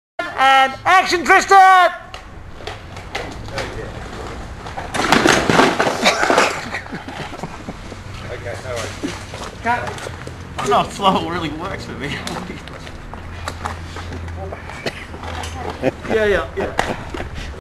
Santa's Stack
Roller blading Santa takes a stack